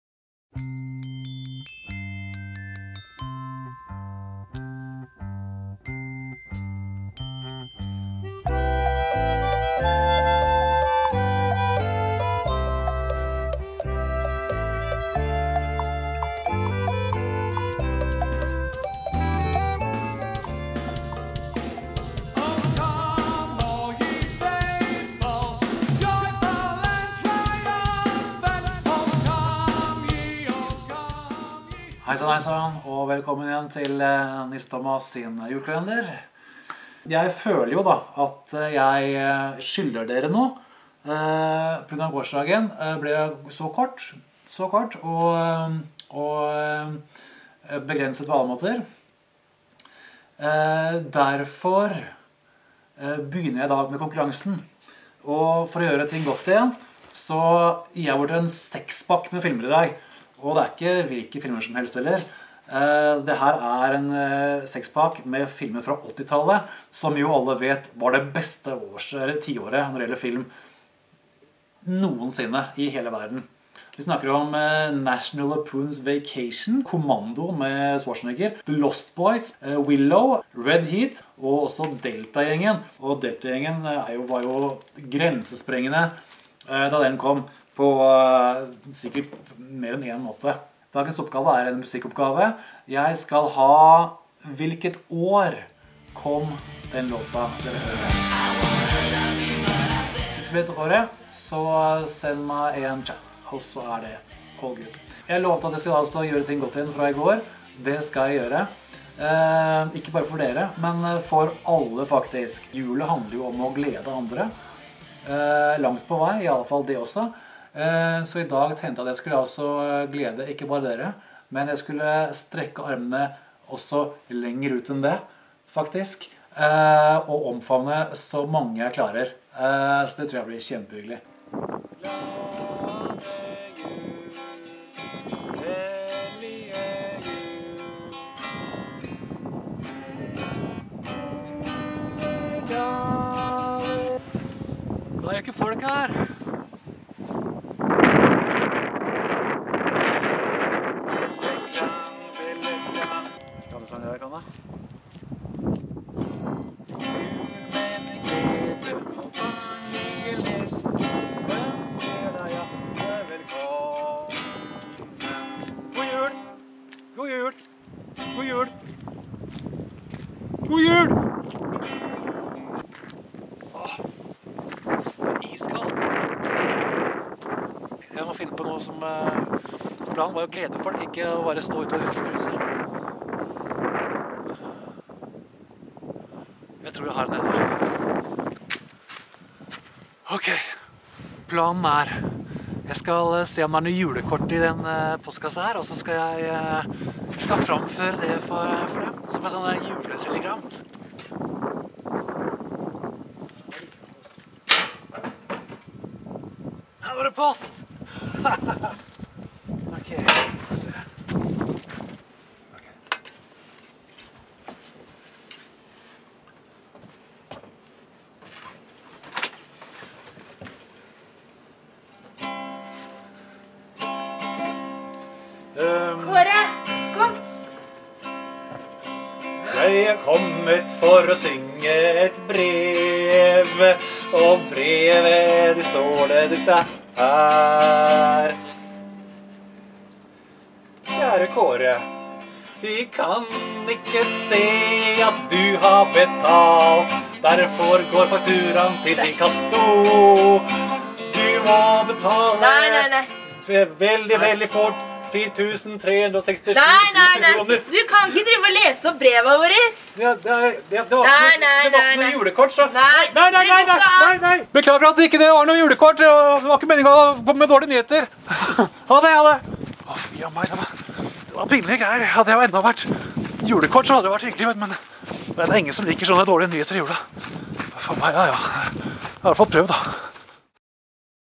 19. desember 2015
NisseThomas har kommet i julestemning, men han vil også gjøre det han kan for å få alle andre i julestemning. Det er sikkert flere måter å gjøre det på. Han velger en av dem.